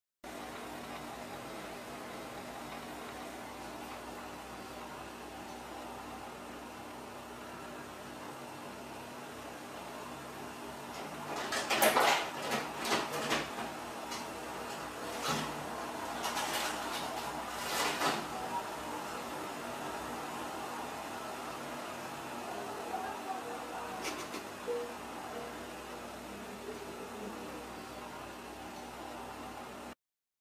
Cat Video : Oitt...What Are You Try To Steal Huh?
This cat try to steal something from the drawer. After get caught, see the face. hahahaha...